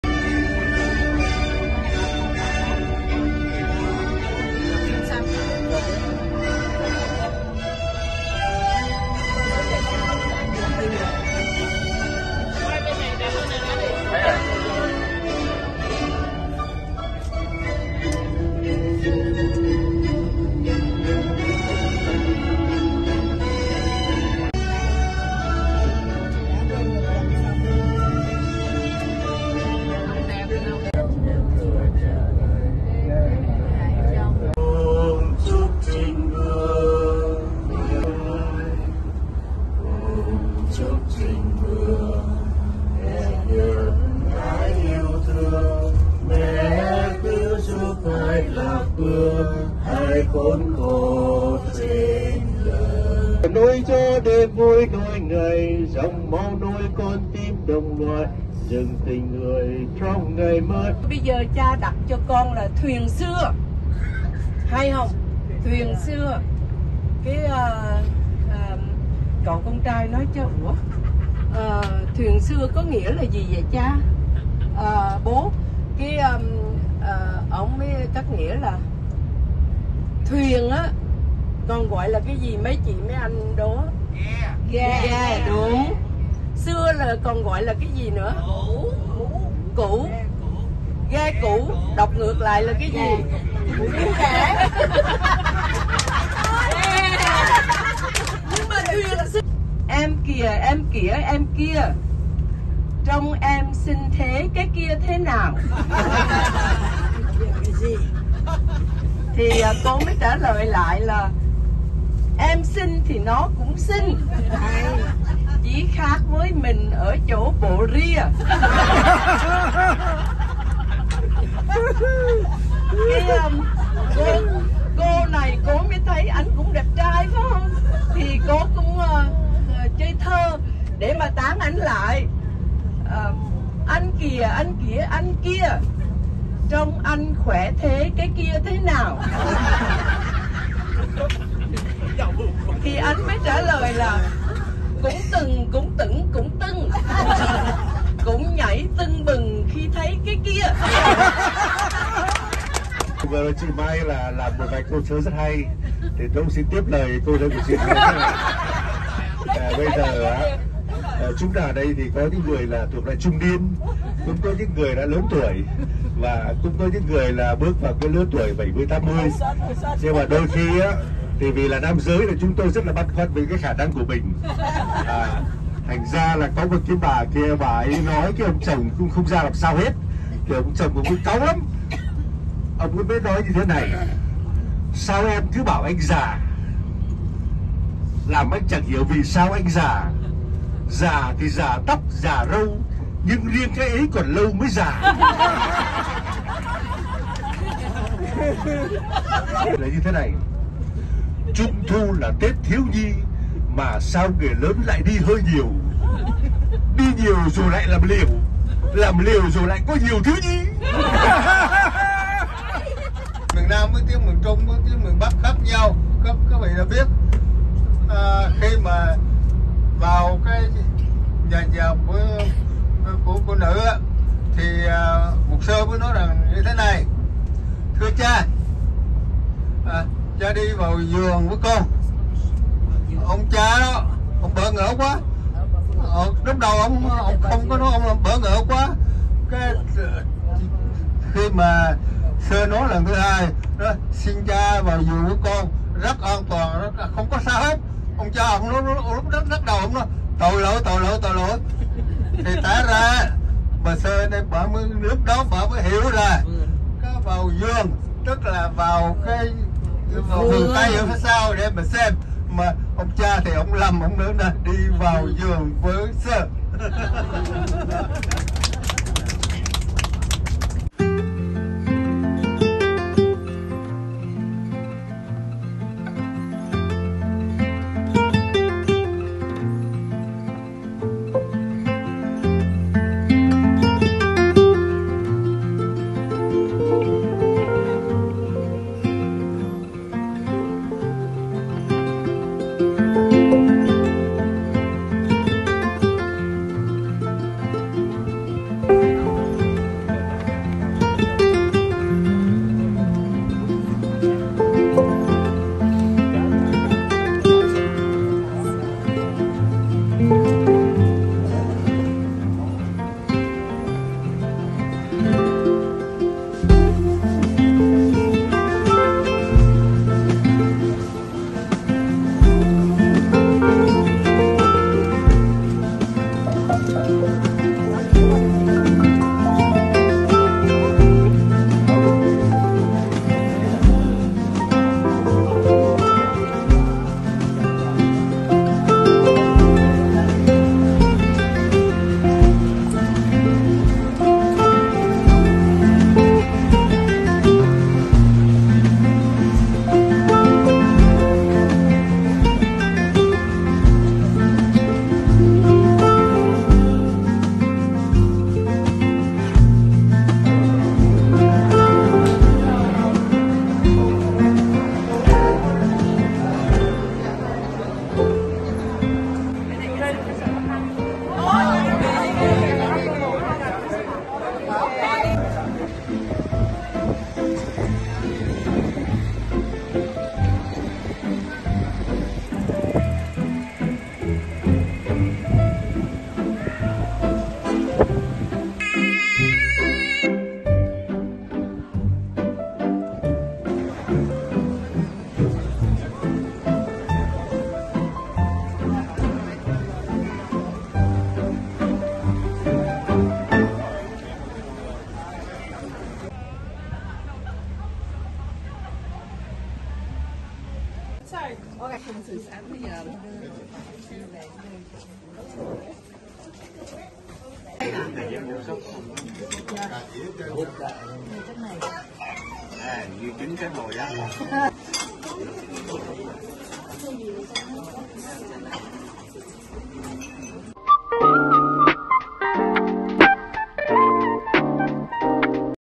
Austria to Germany by bus
Our tour bus take us from Vienna to Munich - similar view to Seattle